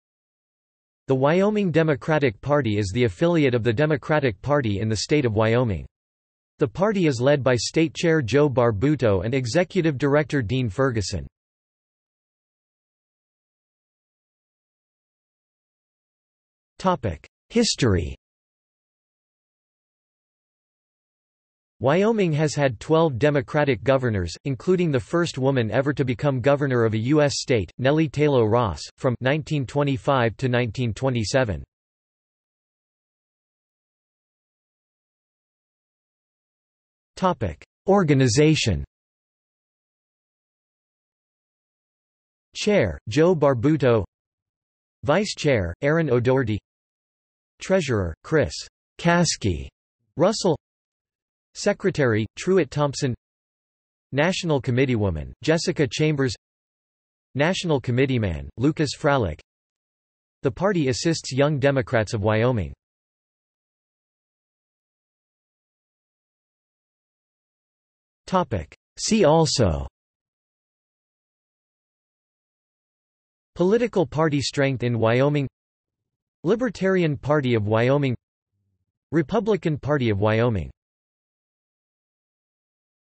Wyoming Democratic Party | Wikipedia audio article
This is an audio version of the Wikipedia Article:
Wyoming Democratic Party

00:00:13 1 History
00:00:33 2 Organization
00:01:04 3 See also

- Socrates